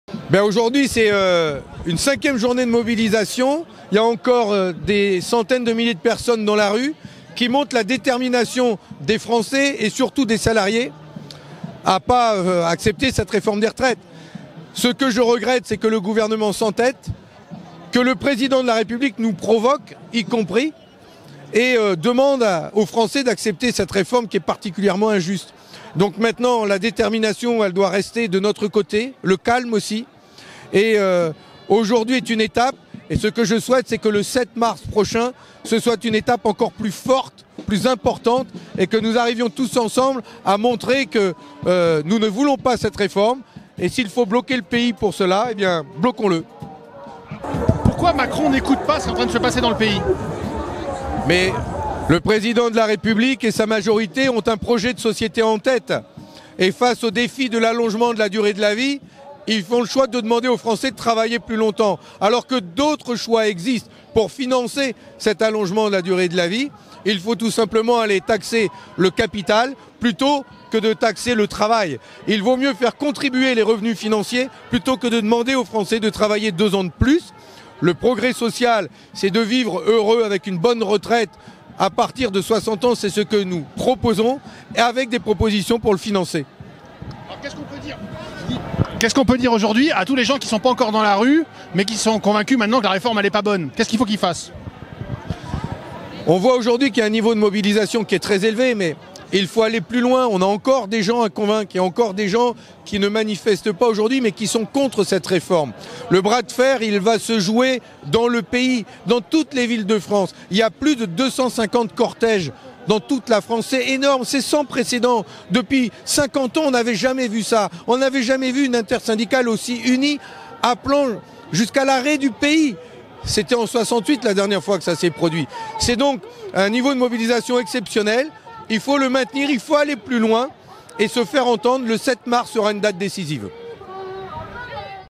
Le gouvernement s'entête, le Président de la République nous provoque avec cette réforme si injuste
Fabien Roussel à la manifestation parisienne contre la réforme des retraites